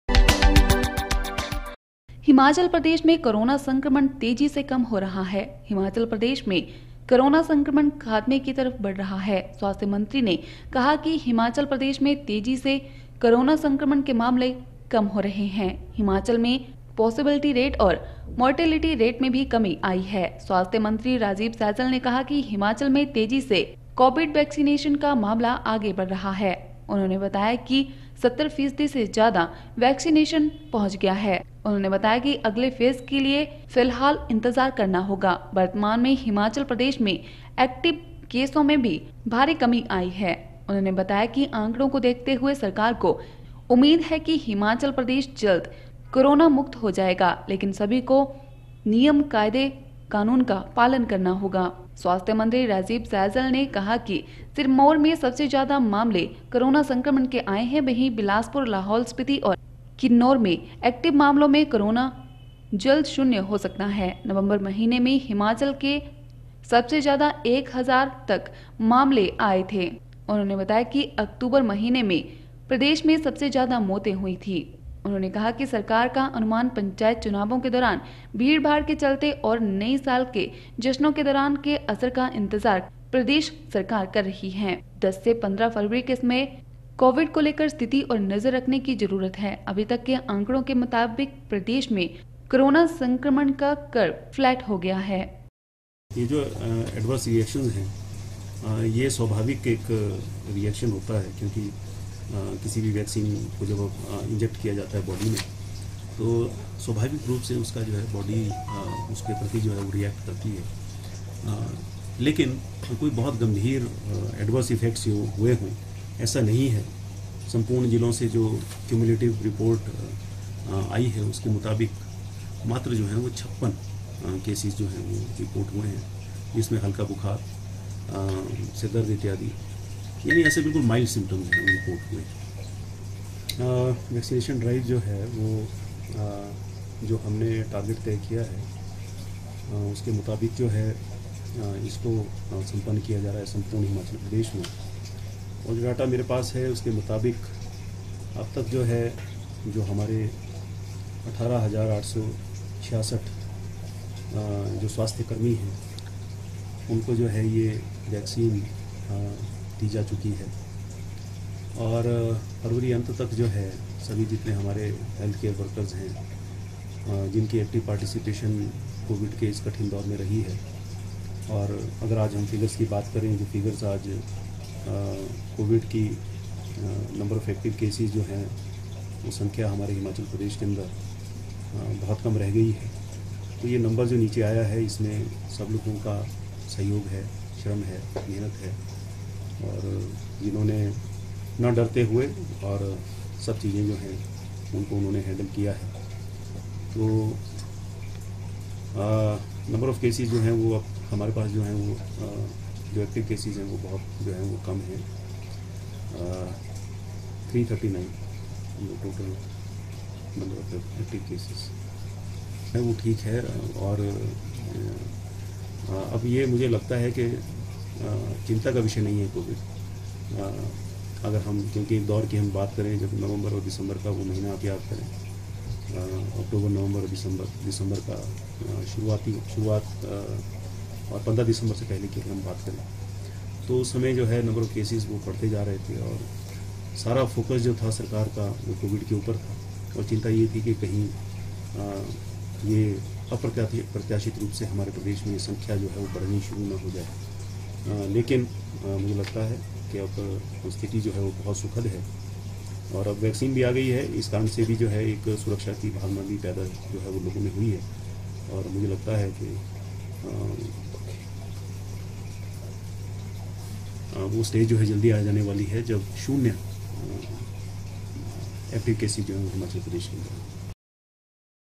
25 JAN 7 स्वस्थ्य मंत्री ने कहा हिमाचल प्रदेश में तेज़ी से कम हो रहे है संक्रमण के मामले।
स्वस्थ्य मंत्री ने कहा हिमाचल प्रदेश में तेज़ी से कम हों रहें है संक्रमण के मामले।